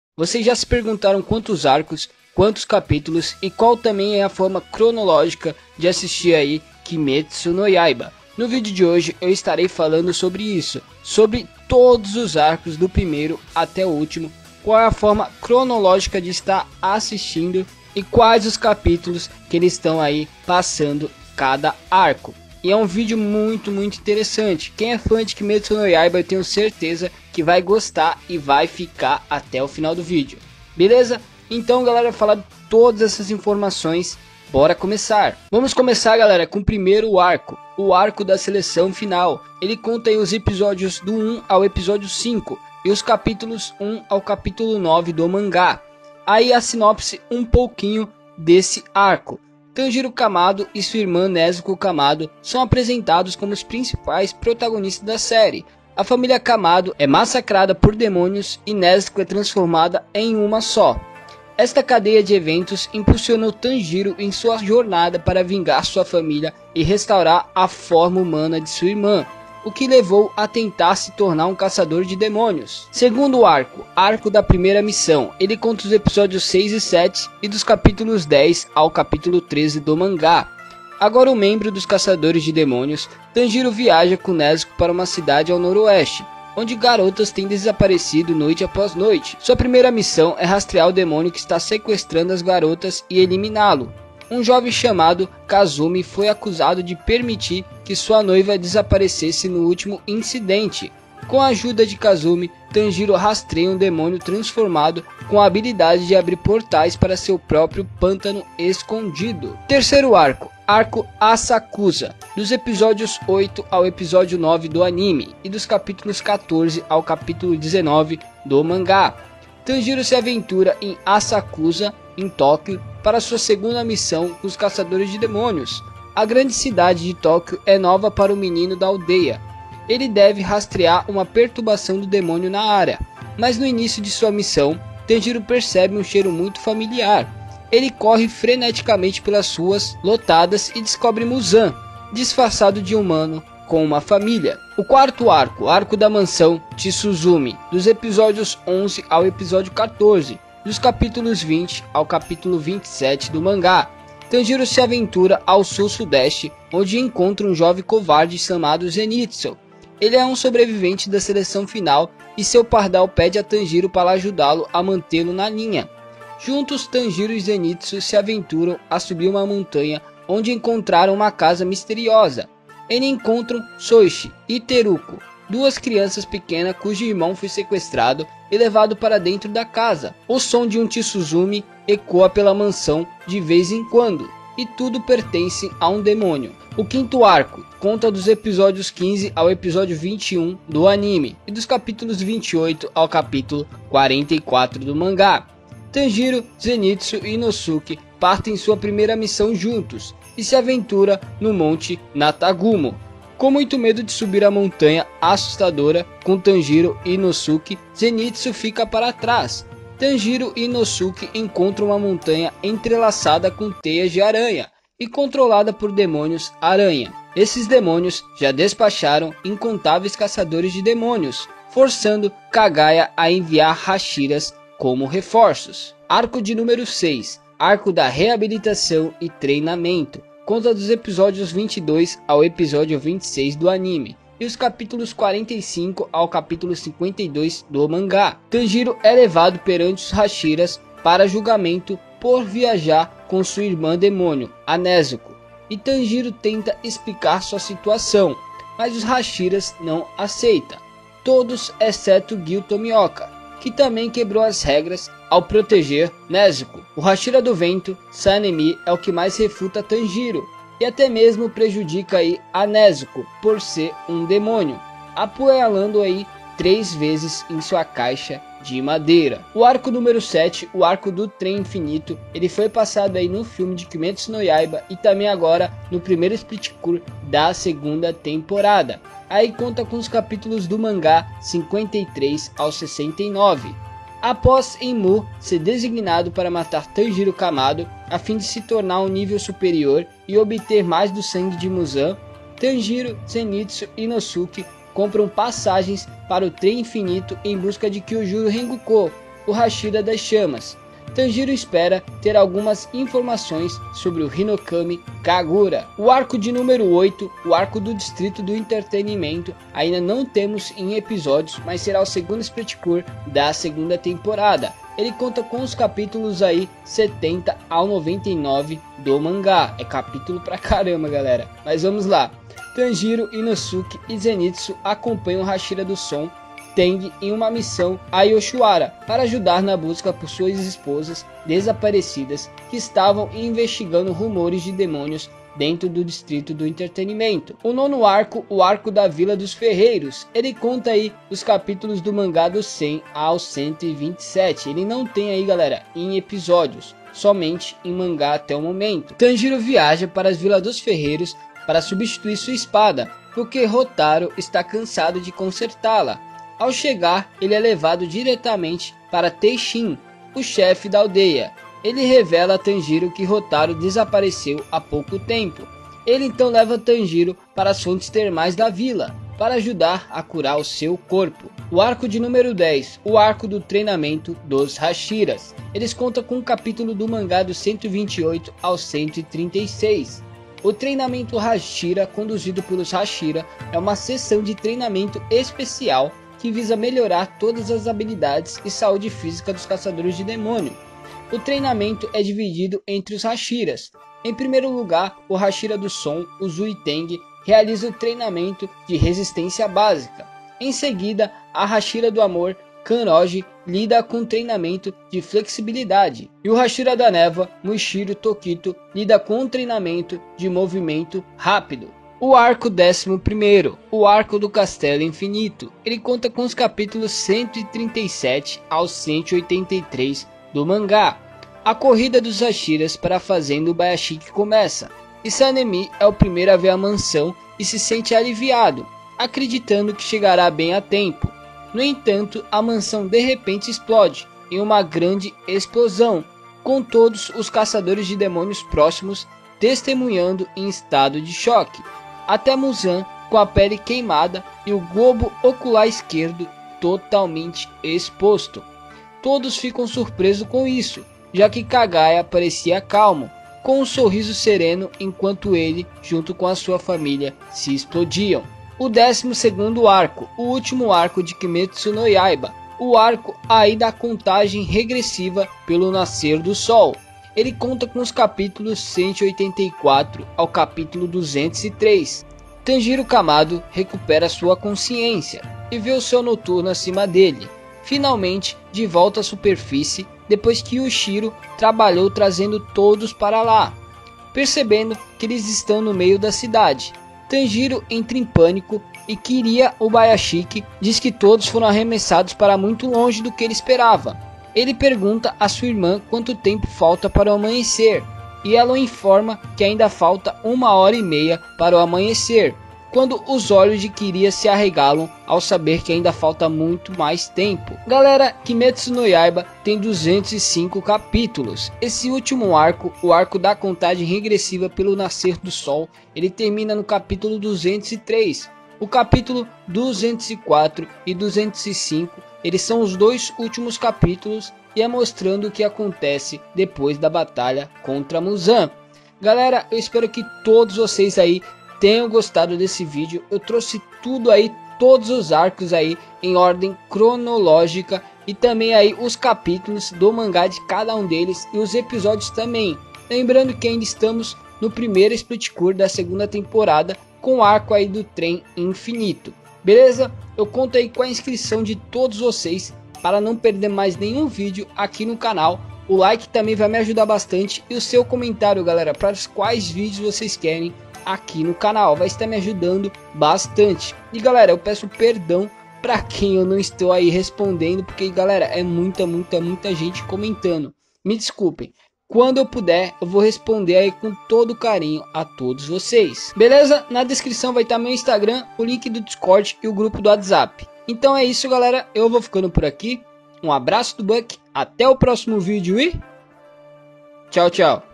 TODOS OS ARCOS DE KIMETSU NO YAIBA DE FORMA CRONOLÓGICA | [DEMON SLAYER]
Veja Todos os ARCOS de DEMON SLAYER de forma CRONOLÓGICA. Nesse vídeo vamos falar de todos os arcos de demon slayer da 1° temporada e 2° temporada até a última temporada de kimetsu no yaiba. Kimetsu no yaiba tem no total de 12° arcos sobre a histótia de tanjiro kamado e nezuko kamado, então bora vê todos os arcos de kimetsu no yaiba [Demon slayer]

TODOS OS ARCOS DE DEMON SLAYER DE FORMA CRONOLÓGICA | KIMETSU NO YAIBA

DATA DE LANÇAMENTO DO FILME NA NETFLIX - Demon Slayer - Kimetsu no Yaiba - The Movie: Mugen Train